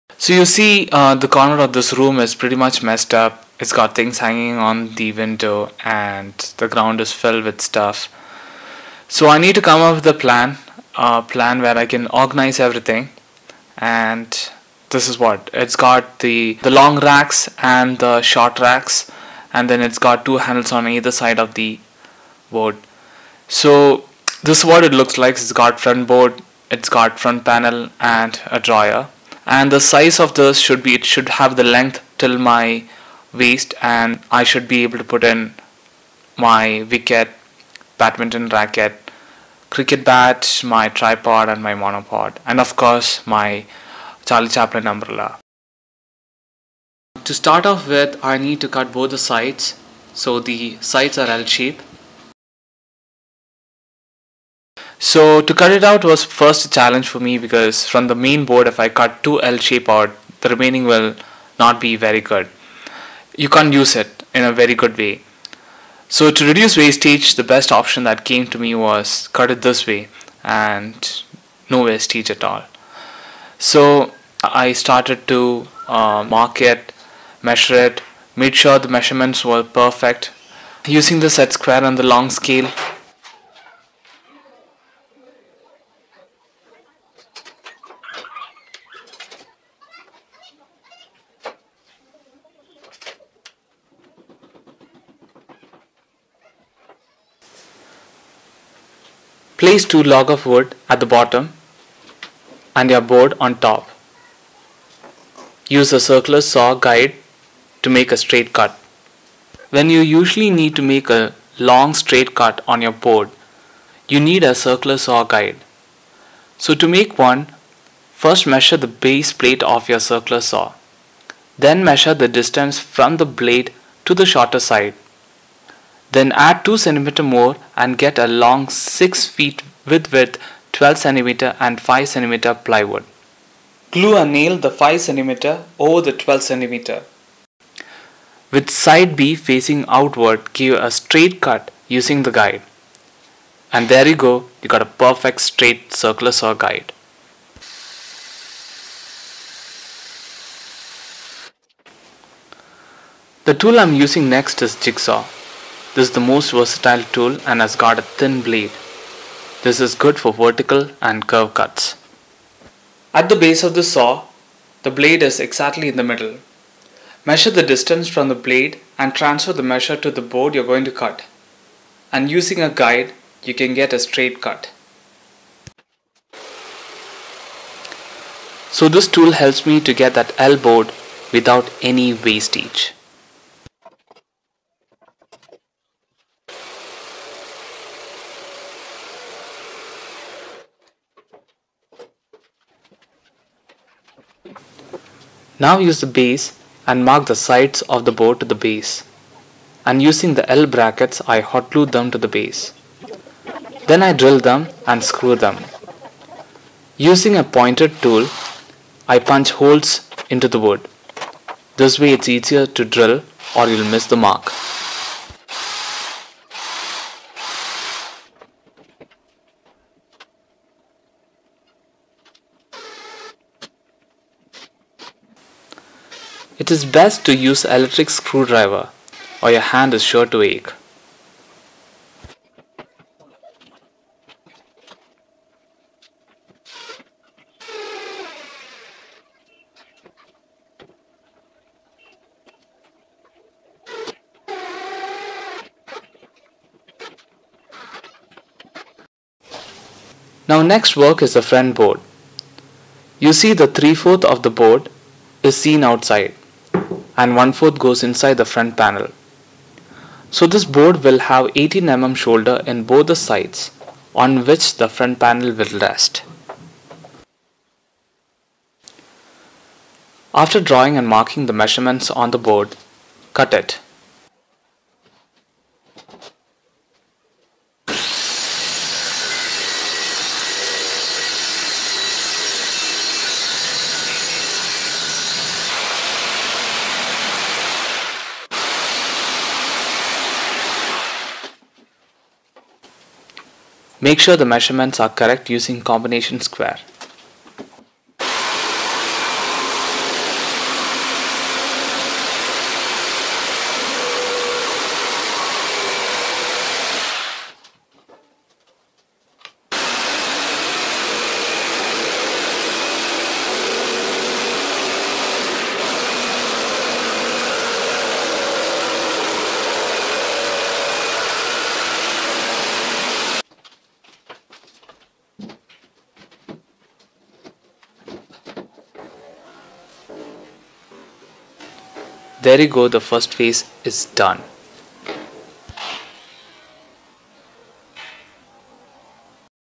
how to make a sports utility cupboard part 1 of 3
here i show how to make a cost effective cupboard for sports equipments. My mission is to teach any layman carpentry if they are interested. need to do a lot of thinking & generate ideas to make it cost effective+less wastage+strong enough+creative free thinking.